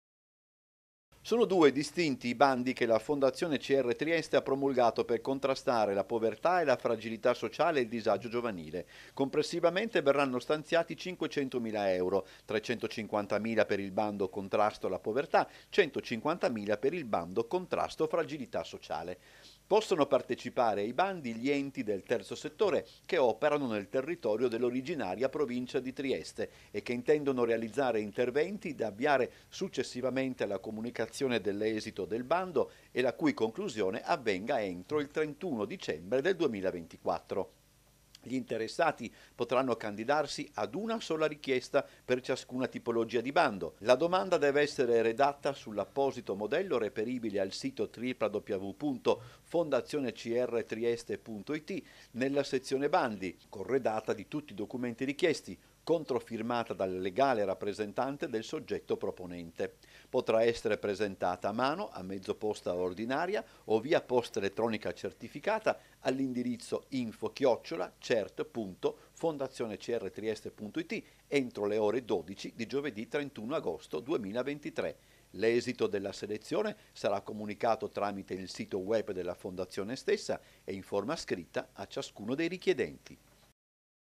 CONTRASTO ALLA POVERTA' E AL DISAGIO: 500MILA EURO DALLA FONDAZIONE CRTRIESTE | 30/06/2023
TG sera 30/06/2023 - TRIESTE - La Fondazione CrTrieste ha istituito due bandi per la lotta alla povertà e al disagio delle famiglie, per uno stanziamento complessivo di 500mila euro. Le domande dovranno pervenire entro il 31 agosto. (Servizio di Umberto Bosazzi) - Segui Telequattro anche sul digitale terrestre! Visita il sito